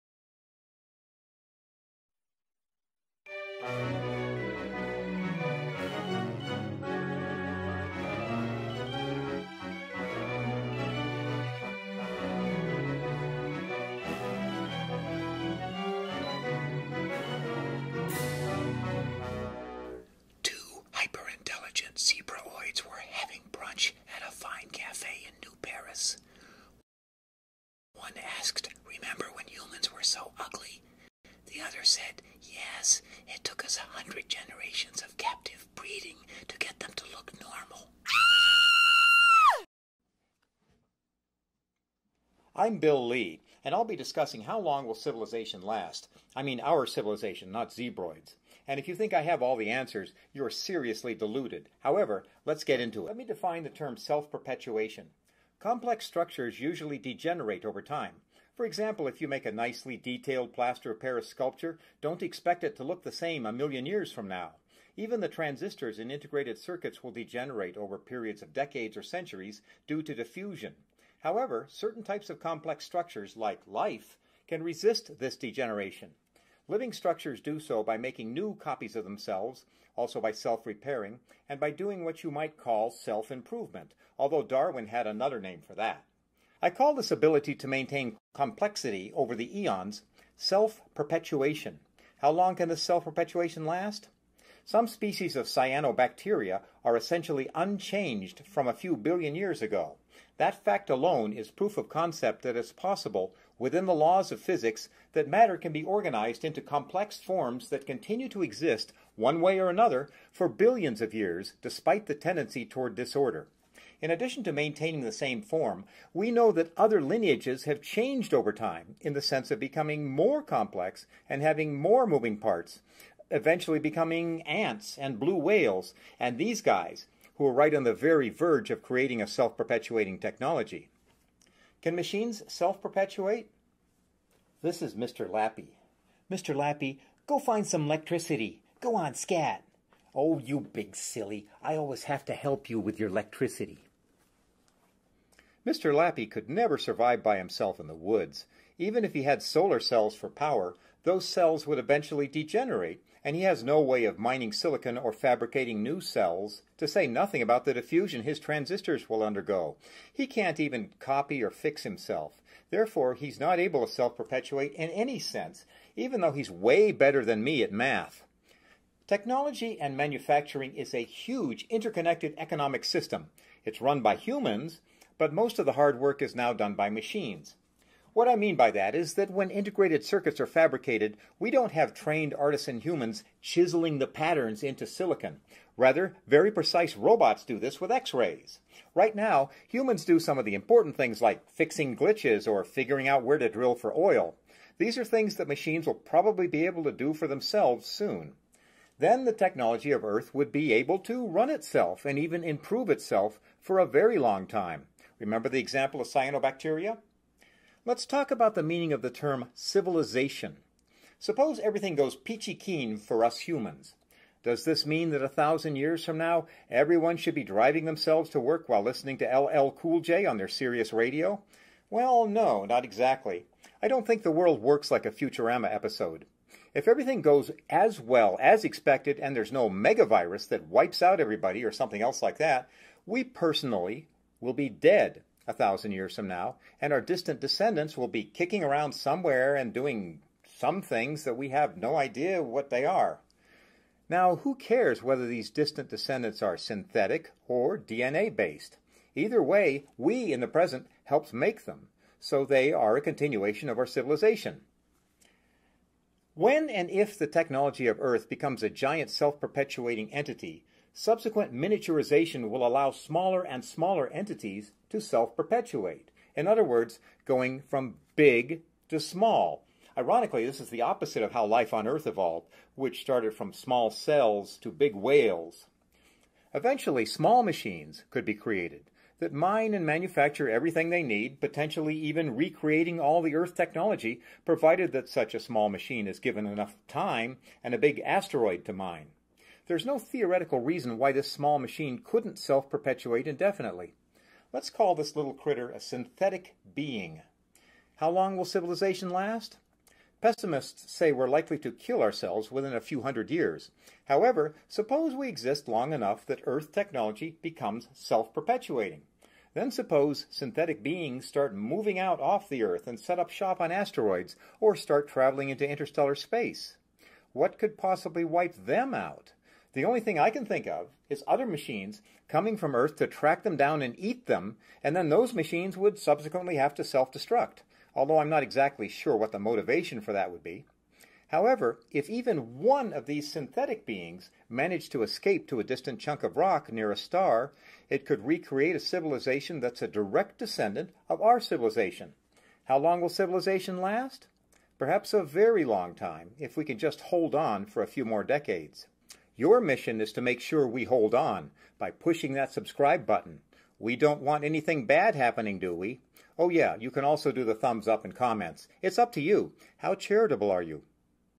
PHYSICS PIT STOP - Episode 2 - End of Our Civilization
physicspitstop robots artificialintelligence Have you ever thought what will End Our Civilization? Will the world consist of Zebra-oids, Robots, and Synthetic Human Beings? PhD Physicist, Bill Lee theorizes about what the future entails for us as a species. Learn about the type of things that could be ahead for us and what will happen to our civilization. Will we all be driving to work listening to LL Cool J like an episode of FUTURAMA or we will not EVOLVE as a species? In this fun filled episode our host, PhD Physicist Bill Lee covers all theories from how future civilizations could evolve and be like on Asteroids to Artificial Intelligence self perpetuating. Now Go On Scat